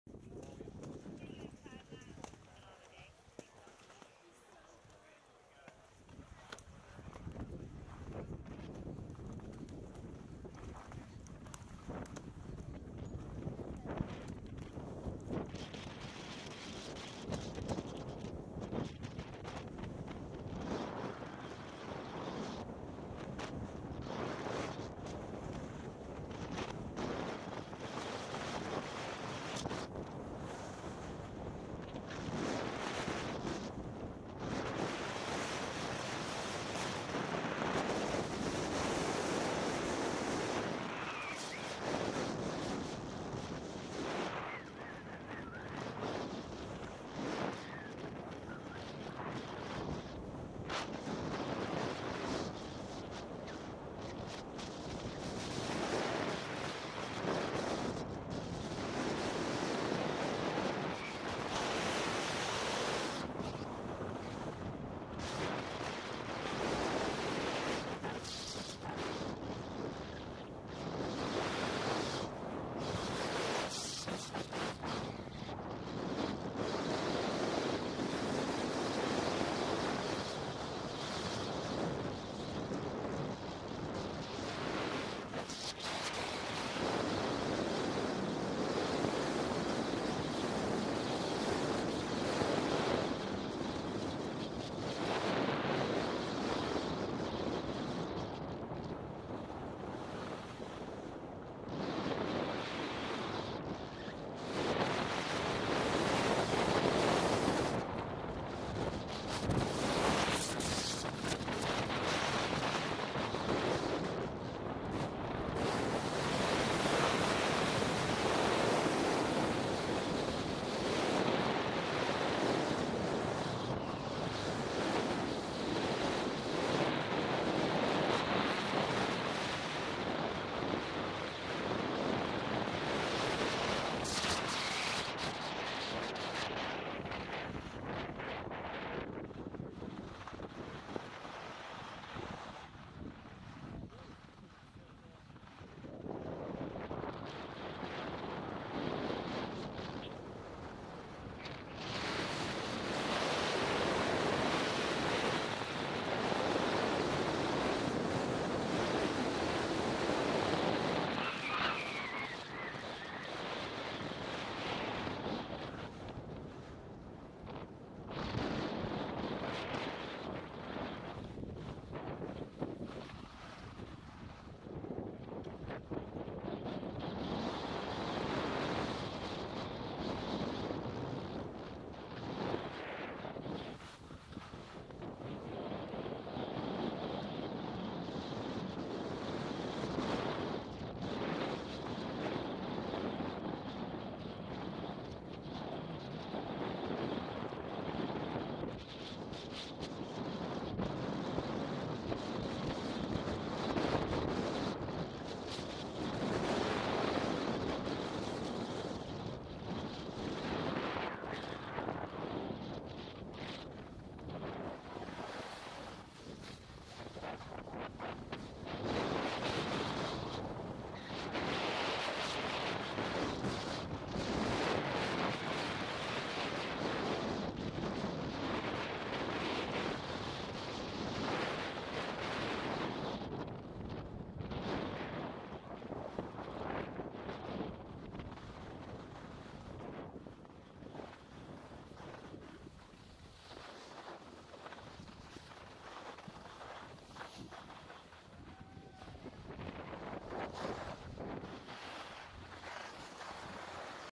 Breckenridge Peak 7 Angel's Rest-Trail 360
A full 4K 360° VR compatible video of Breckenridge Ski Resort. This blue run is located on Peak 7. After turning right off of the Independence SuperChair Lift, Angel's Rest will run along with Monte Cristo for a short time and then fork off to form its own run. Once Angel's Rest ends you will run into the lower half of Monte Cristo and then make your way to the bottom of Independence SuperChair and Peak 7

Start Elevation: 11,449 ft.
Distance: 1.6 mi.
Date: 3/11/2018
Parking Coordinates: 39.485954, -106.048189